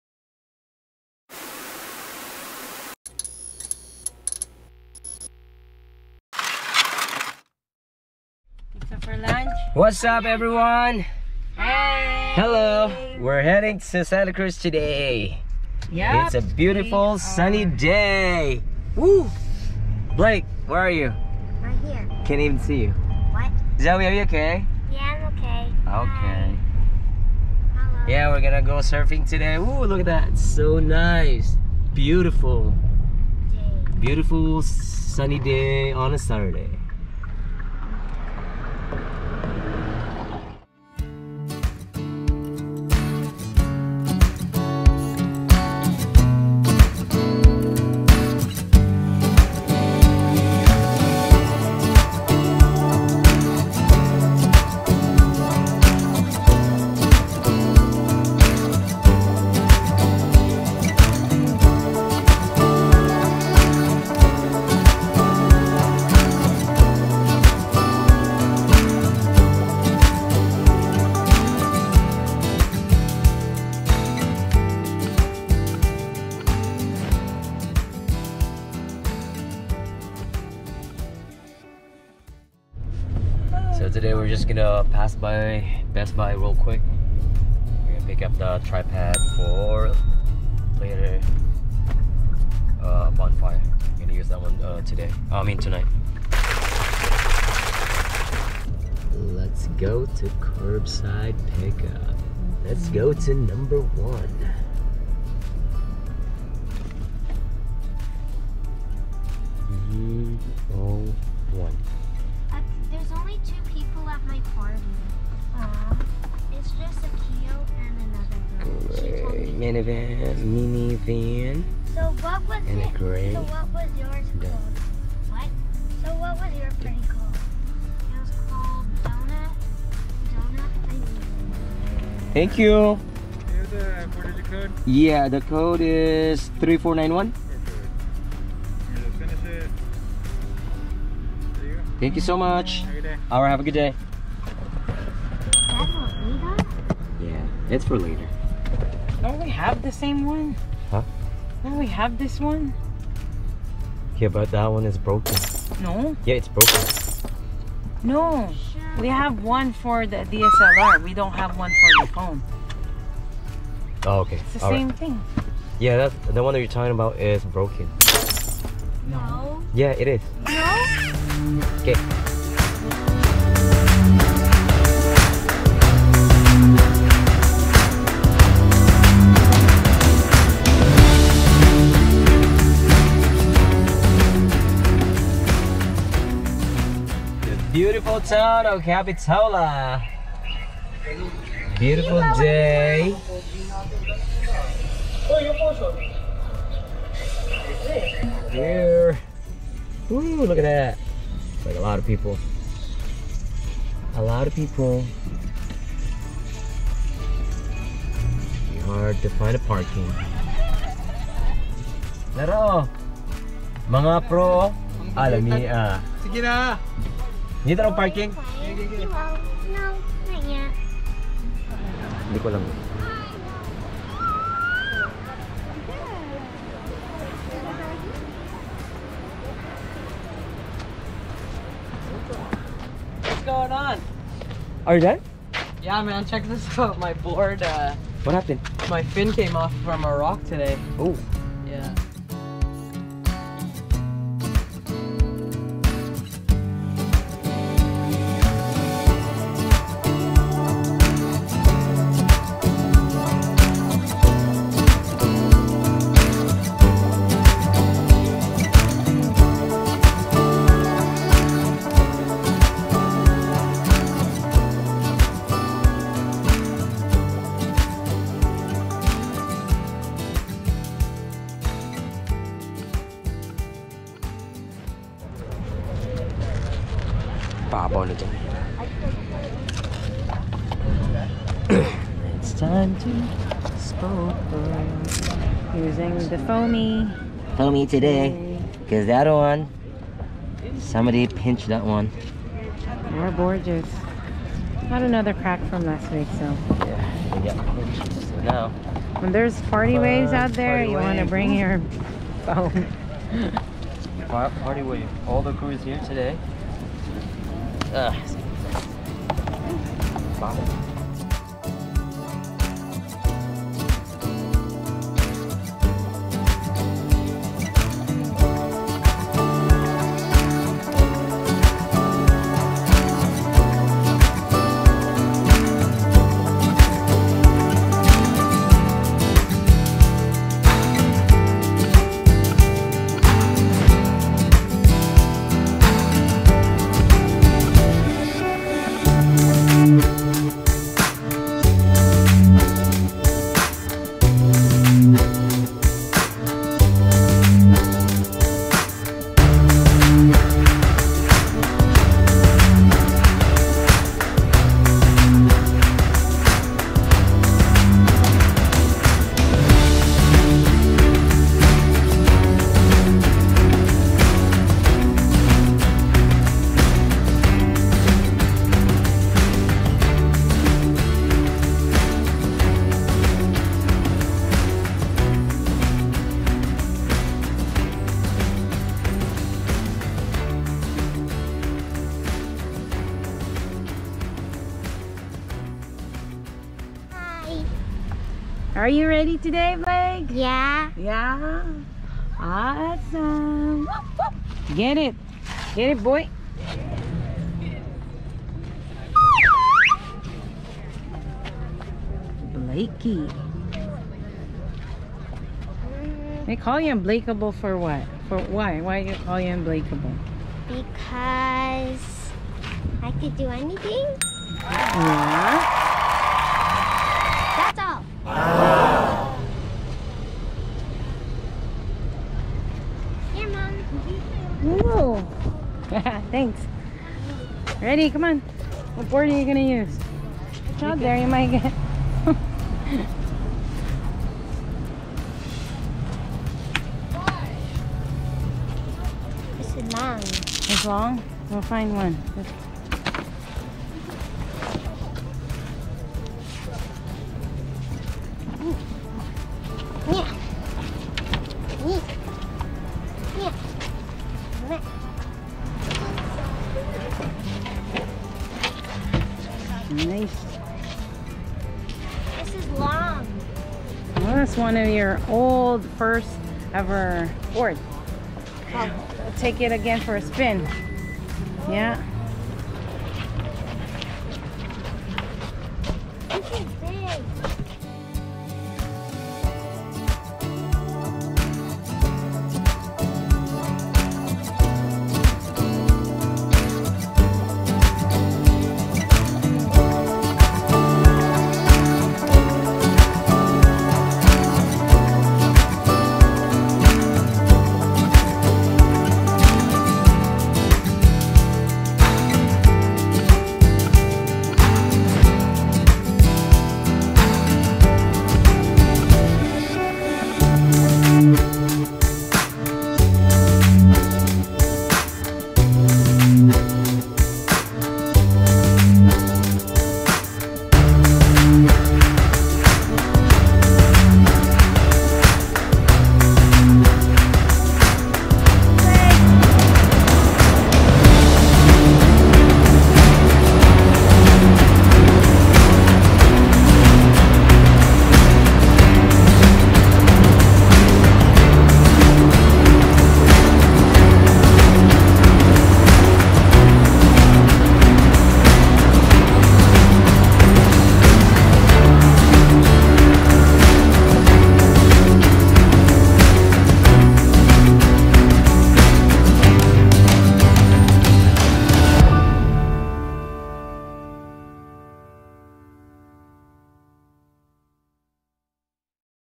Surf and bonfire part 1 + picking up tripod @Best Buy Capitola
Family surf and bonfire with friends @seaside Capitola Part#1